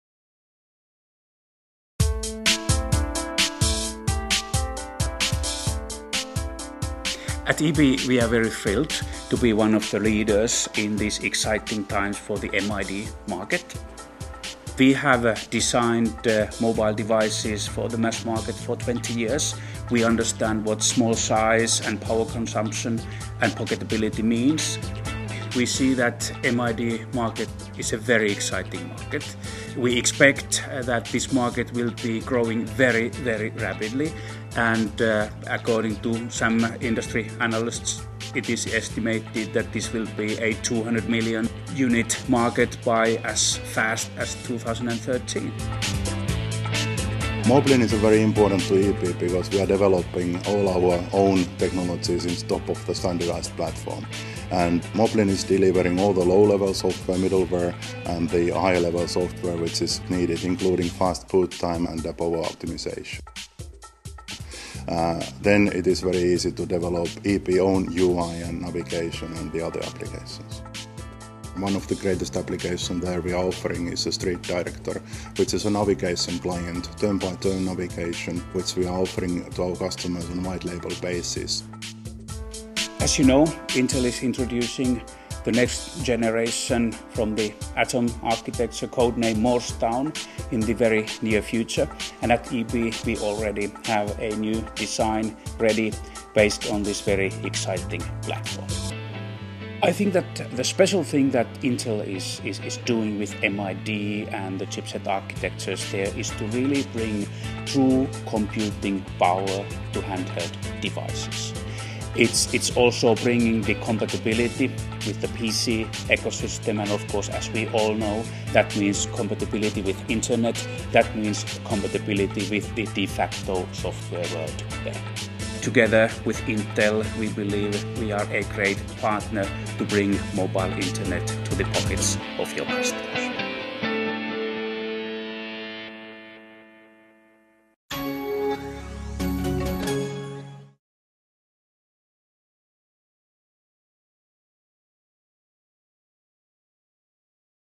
Elektrobit MID video demo
Discover how EB worked with the Intel Atom platform and Moblin regarding their brand new MID reference device and their street director navigation software